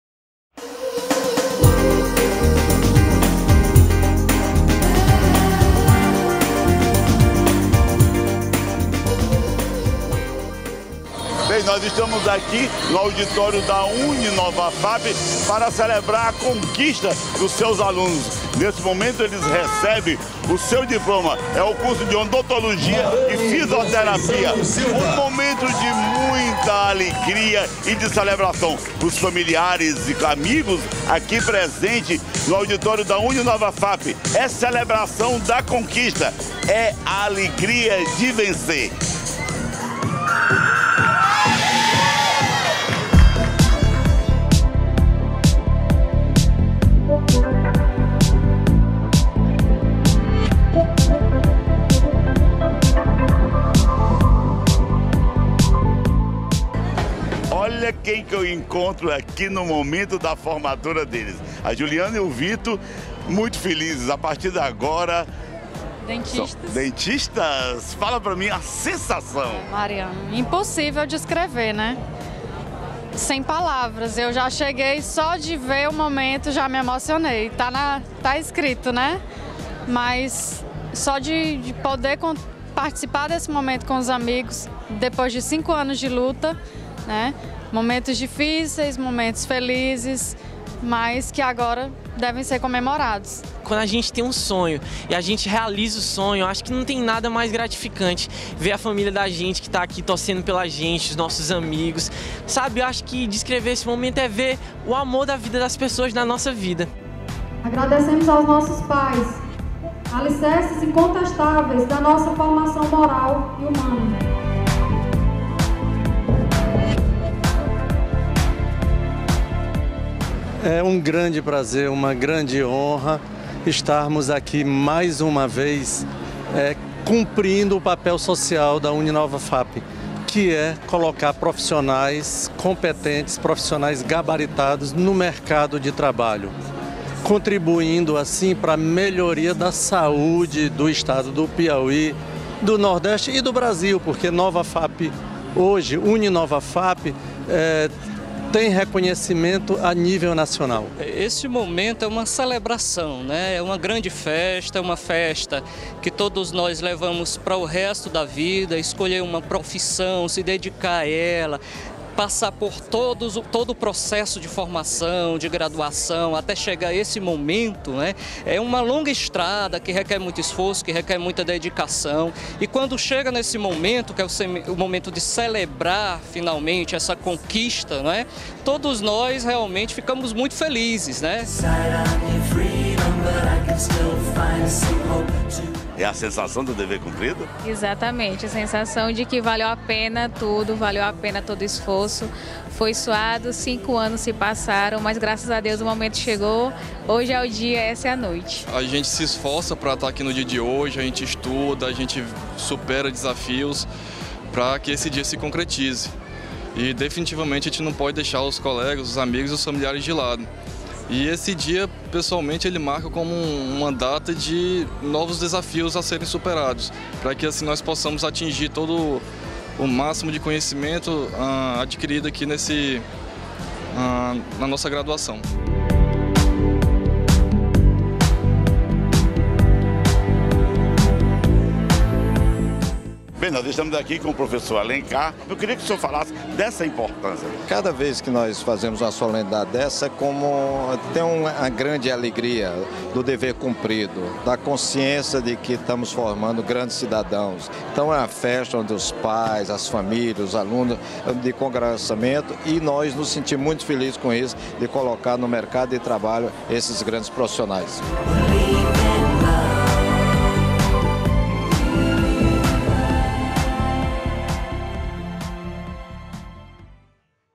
COBERTURA COLAÇAO DE GRAU DA UNINOVAFAPI
Confira a cobertura das colações de Odontologia e Fisioterapia da Uninovafapi, realizada na última quinta-feira, 31 de julho